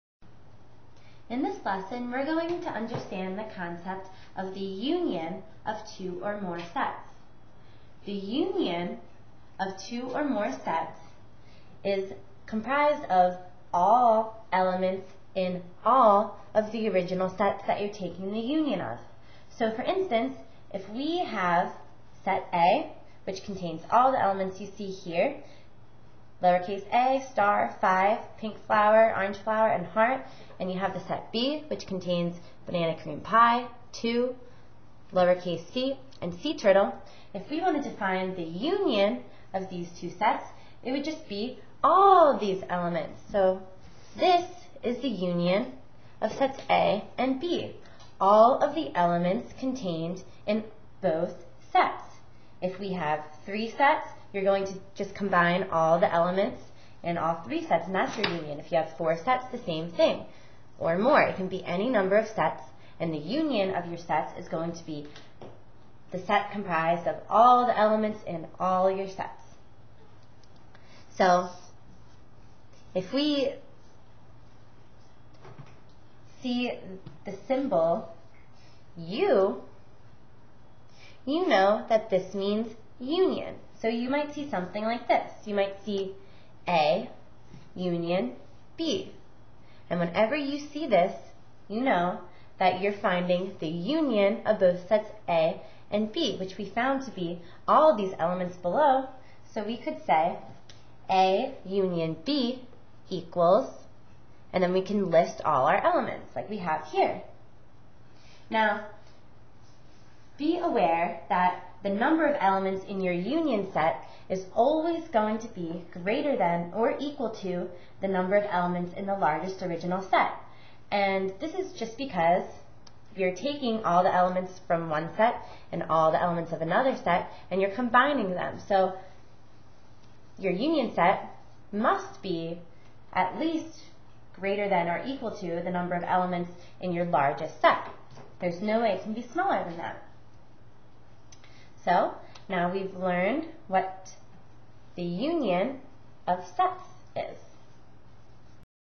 Guaranteach Math Tutorial: The Union of Sets
A short video tutorial from Guaranteach on the Union of Sets.